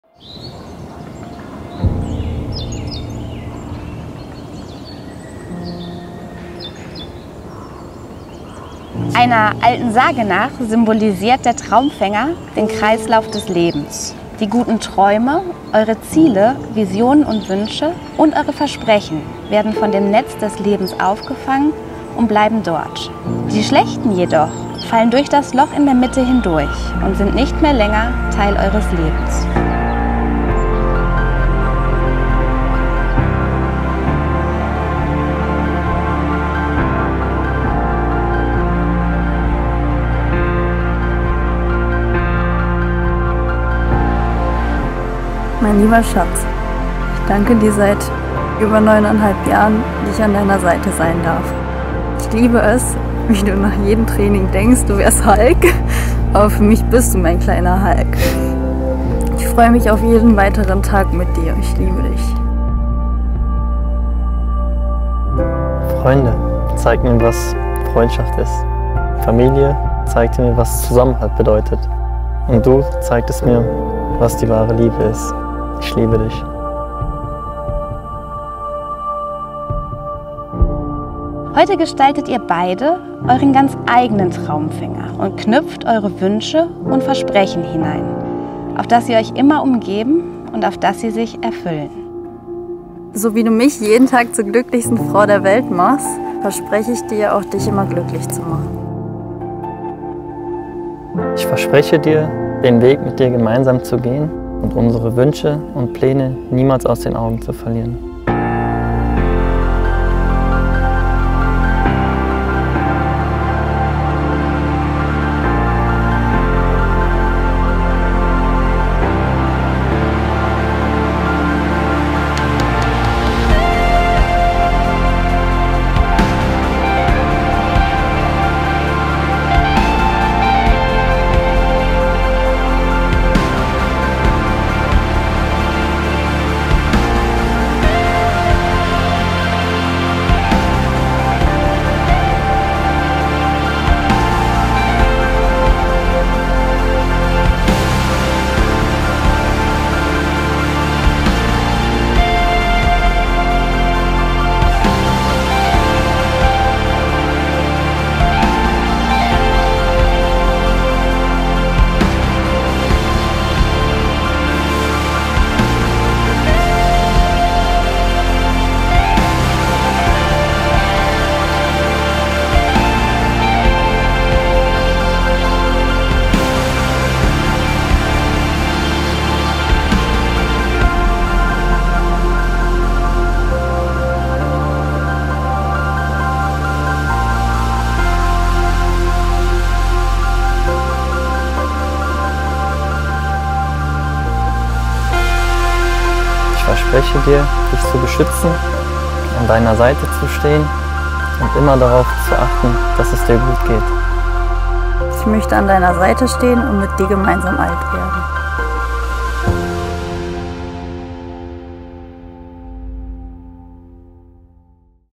Freie Trauung mit Traufräulein im Boho Stil
Heute zeigen wir euch eine wunderschöne Hochzeit im Boho Stil. Die freie Trauzeremonie fand mitten im Wald statt und als Trauritual hat das Brautpaar einen eigenen Traumfänger geknüpft. Bei diesem Style-Shooting haben wir auch bewußt das Thema Makramee aufgegriffen. Außerdem wurde die freie Trauung durch Alpakas begleitet, seht selbst.

Makramee I Miriam Stricker

Hat euch unsere Video zum Thema Boho Hochzeit im Wald gefallen oder habt ihr Fragen zu Green Wedding Hochzeiten, Trauritualen, Hochzeiten mit tierischen Begleitern oder FreieTrauung allgemein? Nutzt die Kommentare oder nehmt den Kontakt zu uns auf.